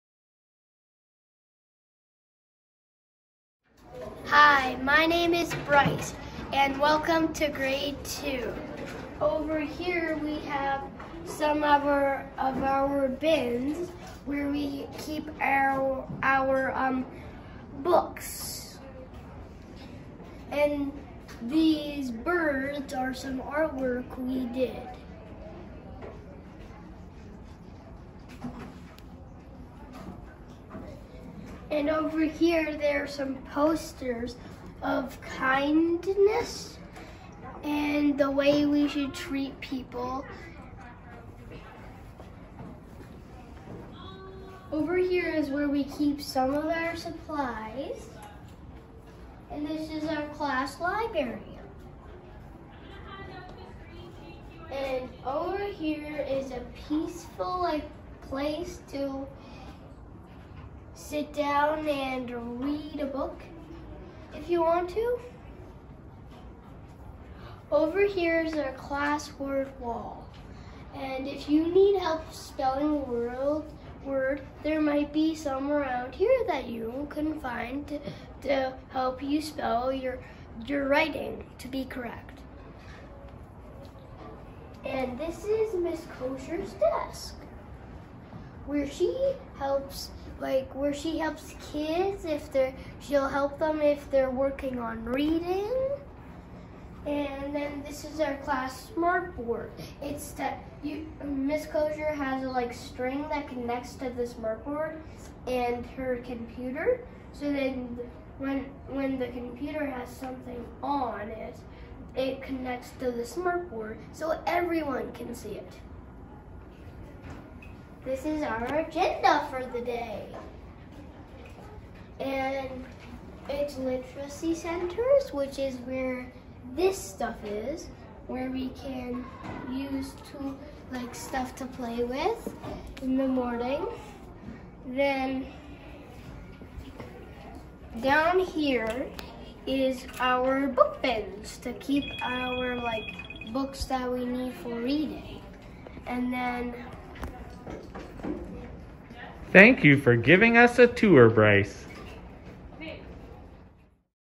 Classroom Tour - Grade 2 - Mrs. Kosior
We invite you to join us on a virtual tour of Mrs. Kosior's grade 2 classroom